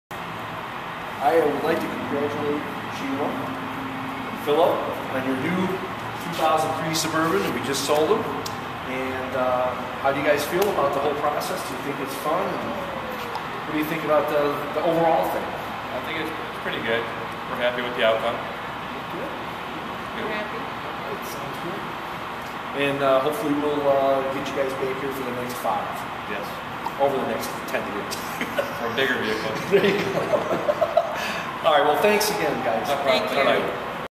Bill Stasek Chevrolet - Testimonial
Phillip and Shiela Lyons purchased a 2003 Chevy Suburban at Bill Stasek Chevy located at 700 West Dundee Rd., Wheeling, IL. or call for more information.